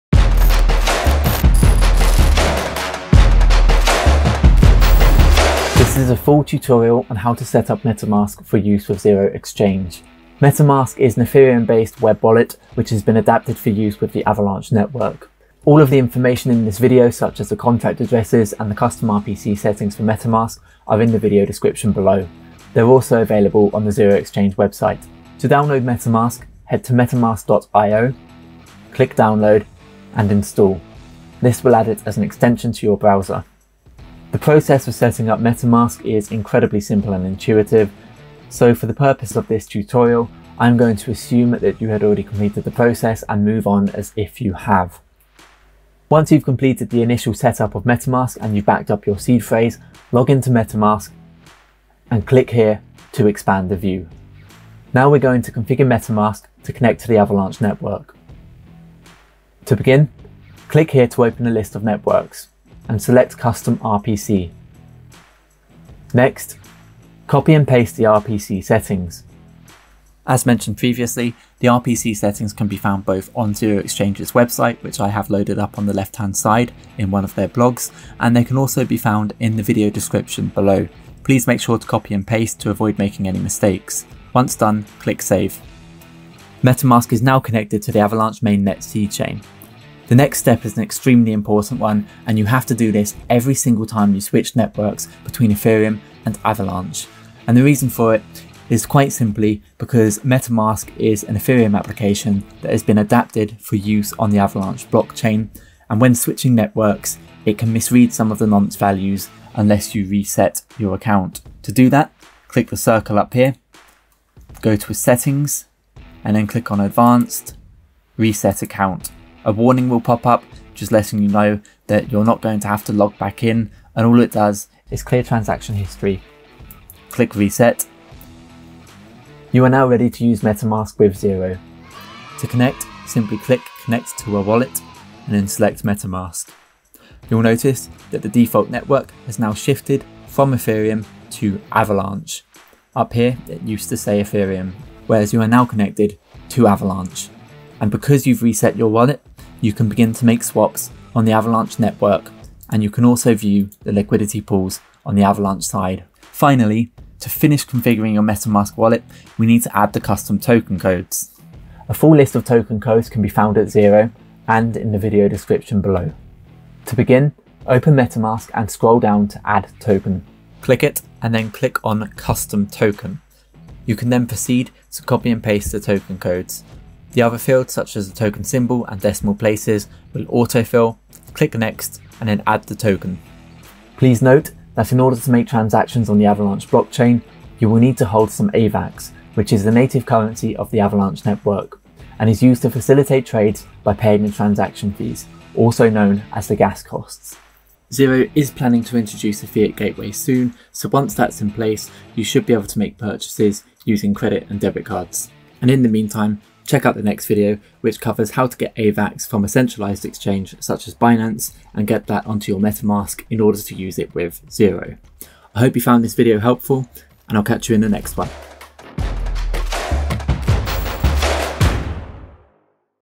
How to configure MetaMask for Zero Exchange | Avalanche Tutorial
Avalanche Mainnet Settings

ChainID: 0xa86a
Symbol: C-AVAX

ARC20 Token Addresses (add custom token in metamask when network chosen is Avalanche):

Address: 0xf6F3EEa905ac1da6F6DD37d06810C6Fcb0EF5183
Name: "zETH"
Decimals: 18

Address: 0x650CECaFE61f3f65Edd21eFacCa18Cc905EeF0B7
Name: "zUSDT"
Decimals: 6

Address: 0x474Bb79C3e8E65DcC6dF30F9dE68592ed48BBFDb
Name: "zUSDC"
Decimals: 6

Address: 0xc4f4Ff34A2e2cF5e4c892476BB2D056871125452
Name: "zBTC"
Decimals: 8

Address: 0x12f108E6138d4A9c58511e042399cF8f90D5673f
Name: "zDAI"
Decimals: 18

Address: 0x008E26068B3EB40B443d3Ea88c1fF99B789c10F7
Name: "ZERO(AVA)"
Decimals: 18

Address: 0xB31f66AA3C1e785363F0875A1B74E27b85FD66c7
Name: "WAVAX"
Decimals: 18